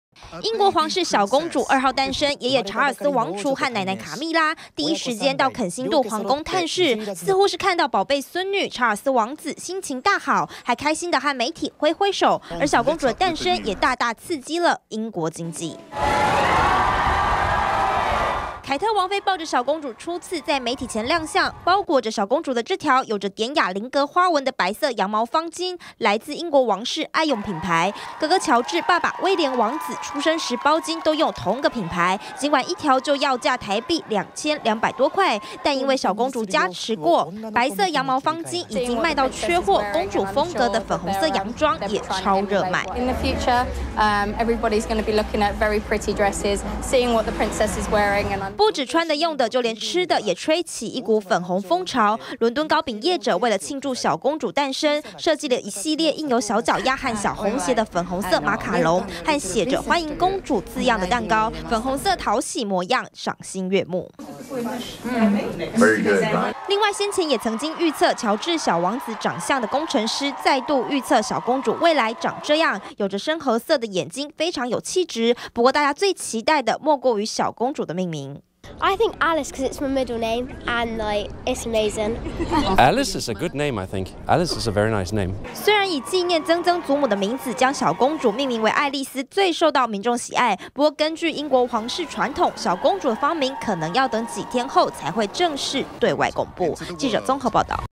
20150504中天新聞 英小公主返家 爺奶外婆登門探視
皇室小公主2號誕生，不只爺爺查爾斯王儲和奶奶卡蜜拉，第一時間都趕往探視，凱特王妃的母親和妹妹，也都到肯辛頓皇宮探望凱特和小公主。英國人最近幾天談論話題離不開小公主，就連凱特幫小公主披上的白色方巾，都熱賣到缺貨，最近還有工程師，用繪圖軟體預測小公主長大後的模樣，眼睛大大的很有氣質！